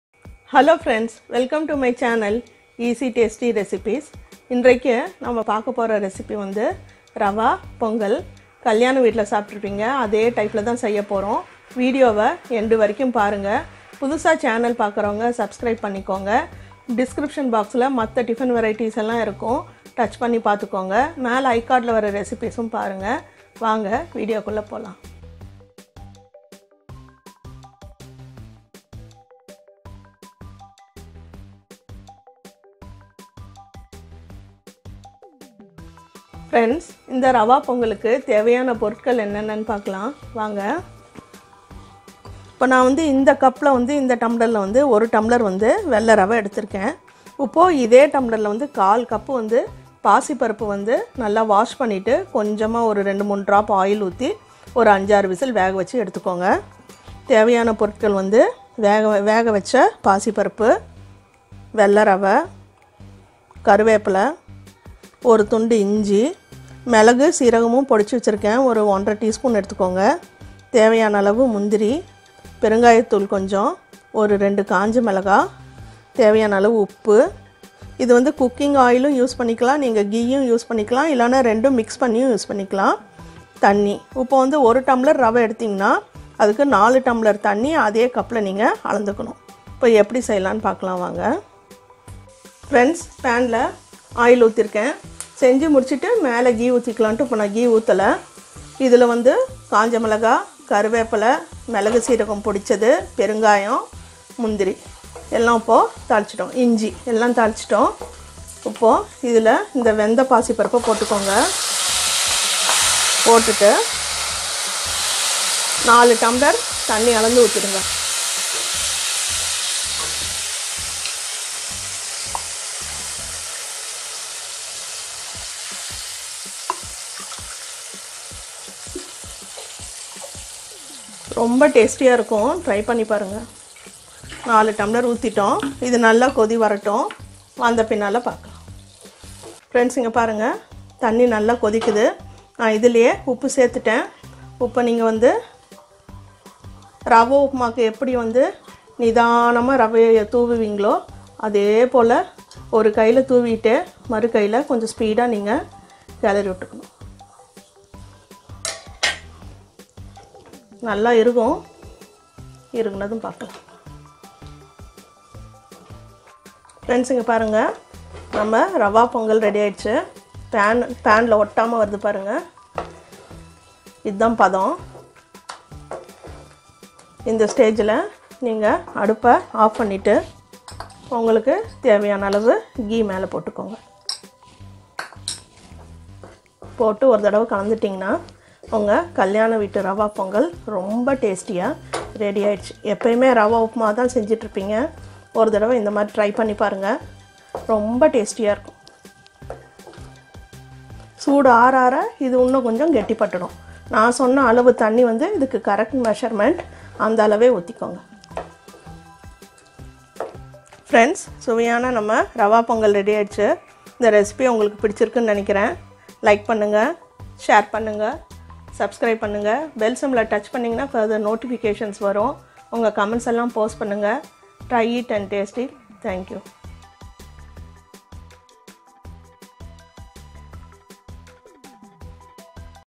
Rava Pongal Recipe in Tamil/How to make Rava Pongal in Tamil
This Kalyana  veetu rava pongal is very tasty and easy to prepare.